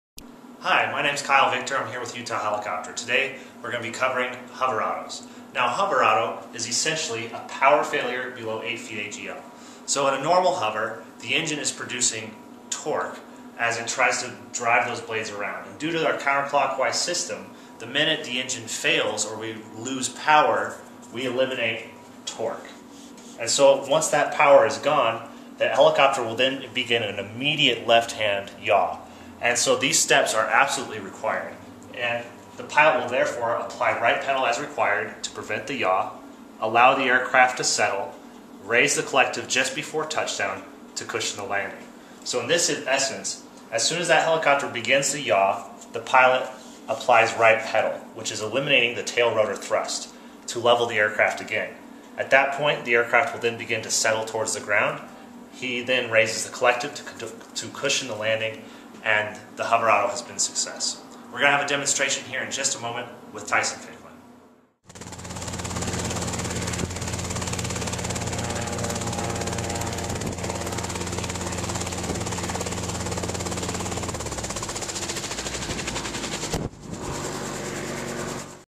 Hover Auto
Utah Helicopter Pilot Kyle Victor teaches us about hover autos.